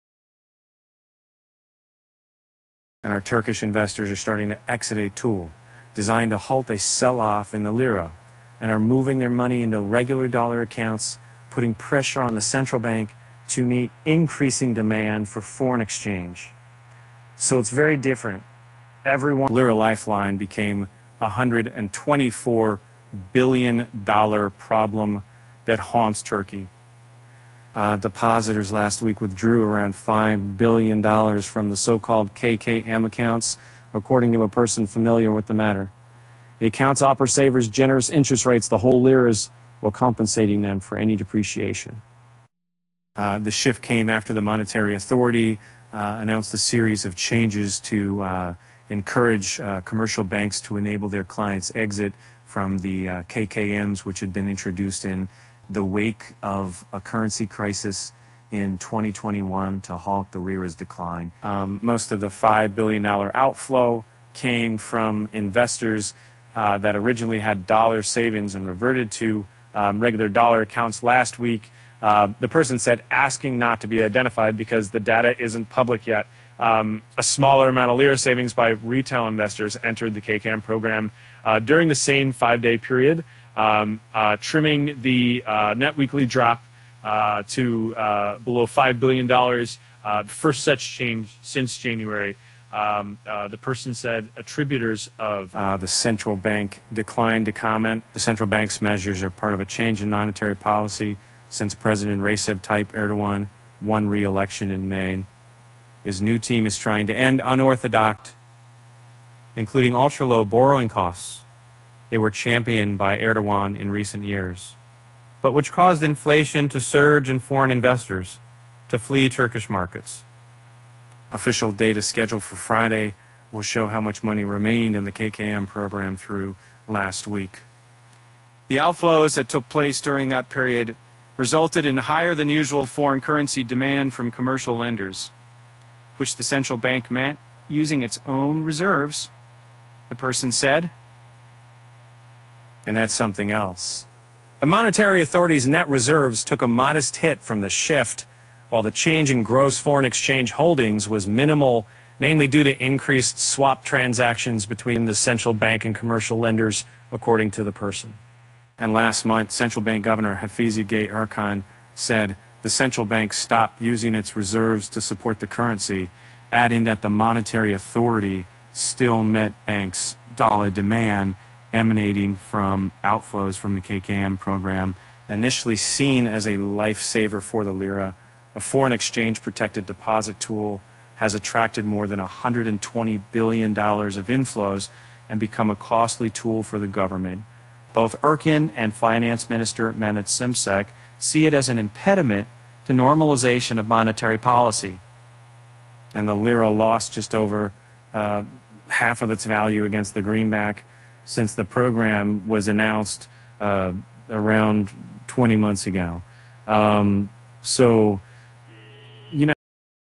Turkish Depositors Exit Key Lira Program as Regulators Start Curbs
Turkish investors are starting to exit a tool designed to halt a selloff in the lira and are moving their money into regular dollar accounts, putting pressure on the central bank to meet increasing demand for foreign exchange.